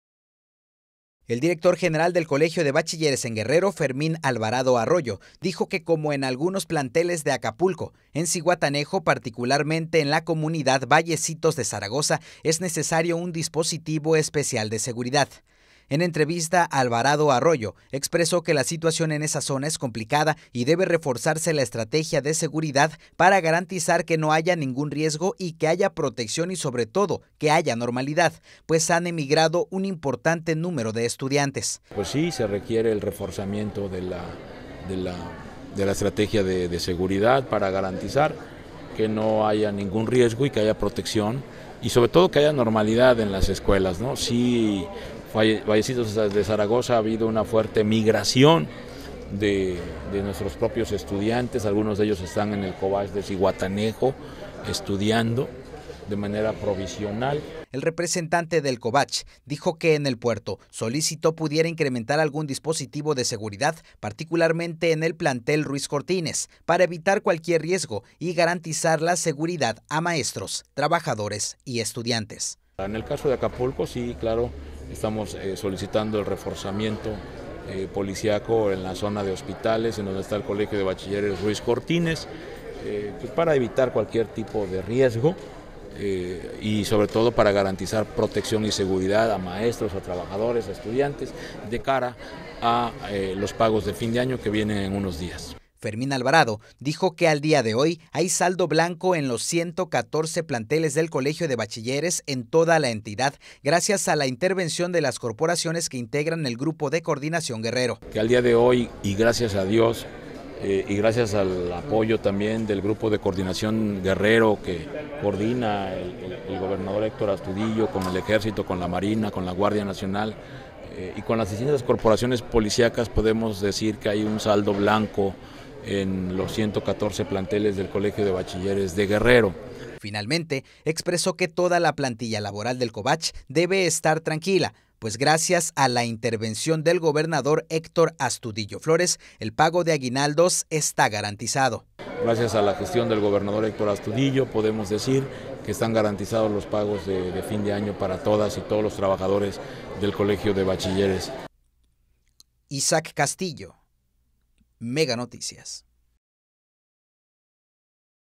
“Cobach Vallecitos” necesita dispositivo especial de seguridad: FAA
El Director General del Colegio de Bachilleres en Guerrero, Fermín Alvarado Arroyo, dijo que como en algunos planteles de Acapulco, en Zihuatanejo, particularmente en la comunidad Vallecitos de Zaragoza es necesario un dispositivo especial de seguridad.